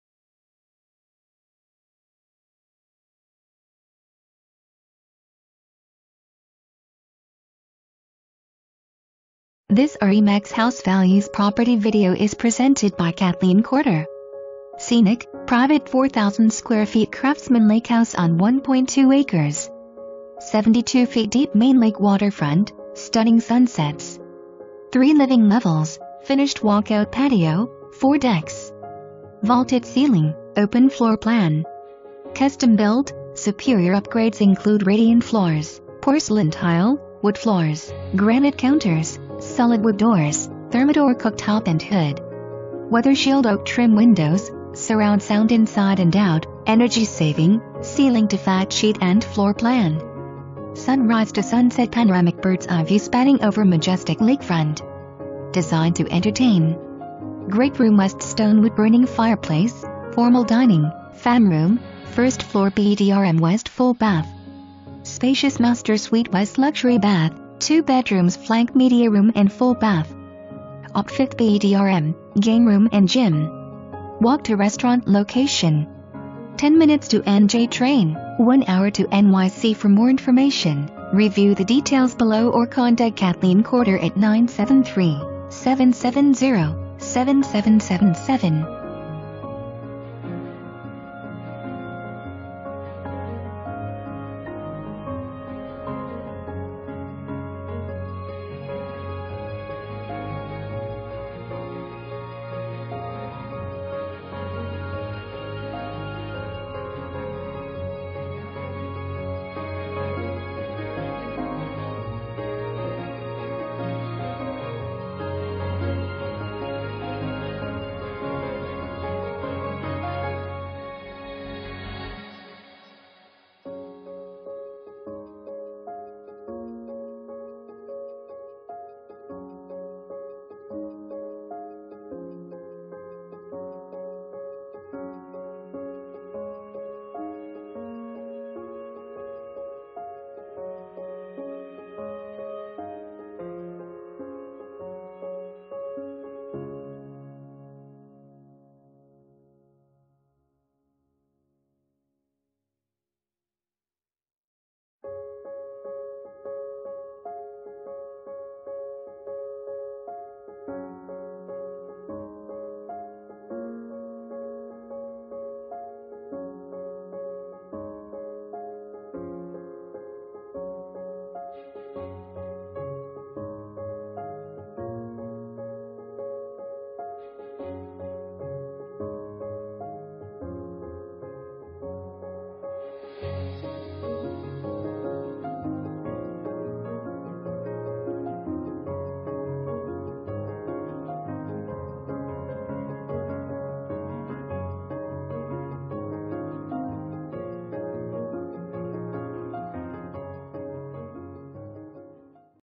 RE/MAX House Values - 41 CASTLE ROCK RD
Please enjoy this property video created by RE/MAX House Values

Scenic, private 4000 sqft Craftsman Lake House on 1.2 acres. 72' deep main lake waterfront, stunning sunsets. 3 living levels, finished walkout patio, 4 decks! Vaulted ceiling, open floor plan. Custom built, SUPERIOR upgrades include radiant floors, porcelain tile, wood flrs, granite counters, solid wood doors, thermador cooktop & hood. Weather shield oak trim windows, surround sound inside & out, energy saving (see link to fact sheet & floor plan). Sunrise to sunset panoramic birds eye view spanning over majestic lakefront. Designed to entertain. Great rm w/stone wood burning fireplace, formal dining, fam rm, 1st floor bedrm w/full bath. Spacious master suite w/luxury bath, 2 bedrooms flank media rm & full bath. opt 5th bedrm, game rm & gym. Walk to restaurant location. 10 min to NJ train, 1 hr to NYC

3 full bath

Kathleen Courter
RE/MAX House Values